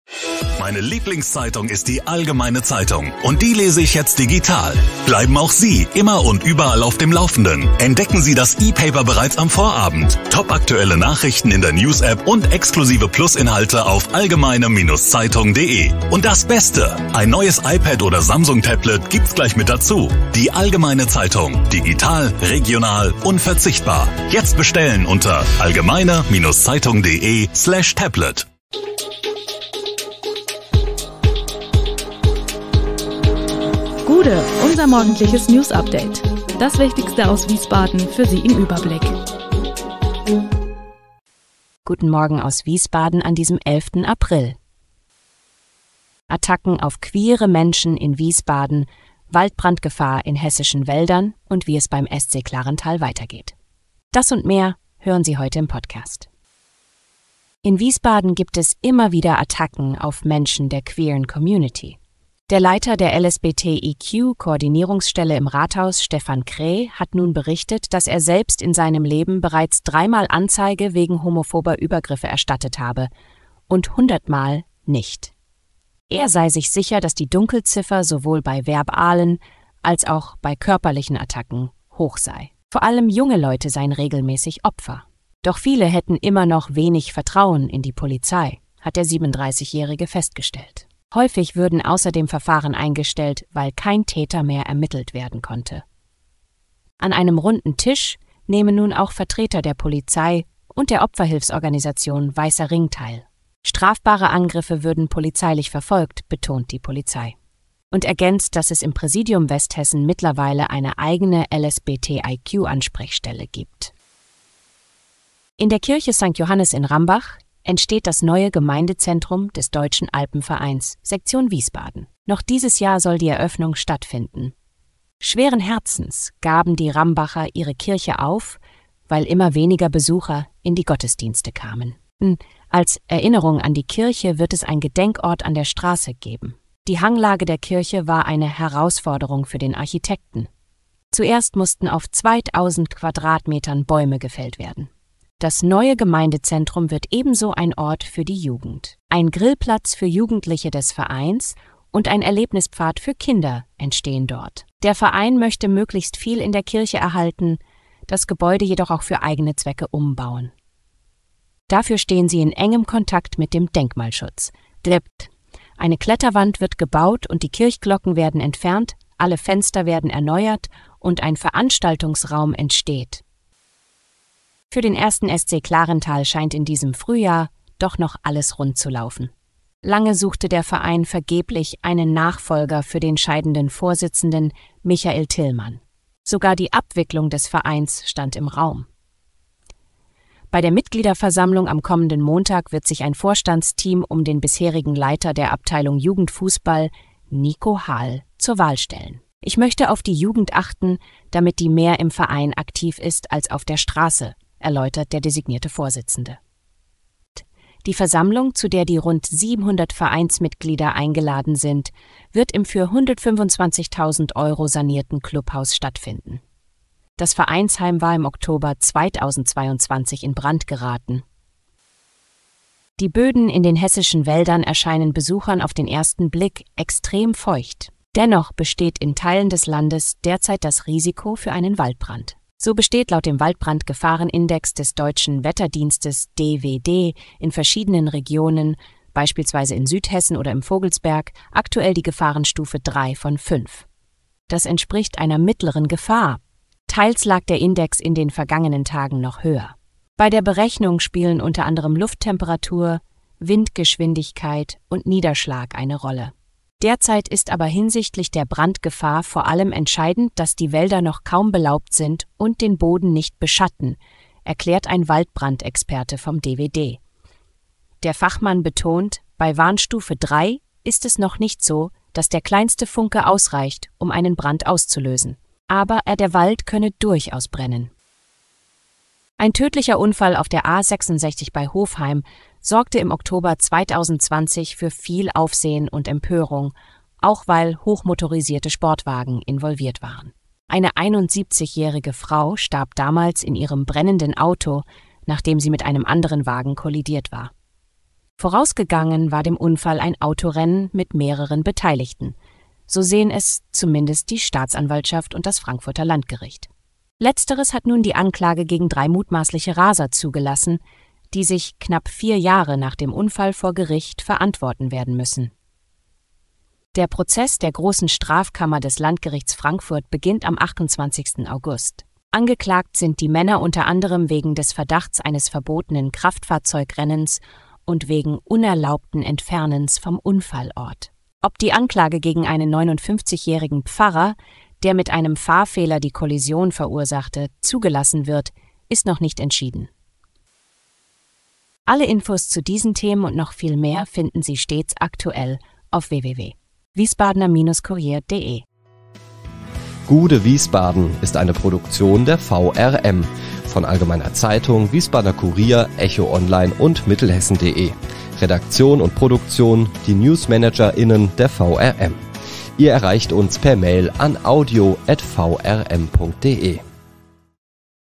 Attacken auf queere Menschen in Wiesbaden - Gude, Wiesbaden!
Attacken gegen queere Menschen in Wiesbaden, Waldbrandgefahr in hessischen Wäldern und wie es beim SC Klarenthal weitergeht. Das und mehr hören Sie heute im Podcast. Alle Hintergründe zu den Nachrichten des Tages finden Sie hier: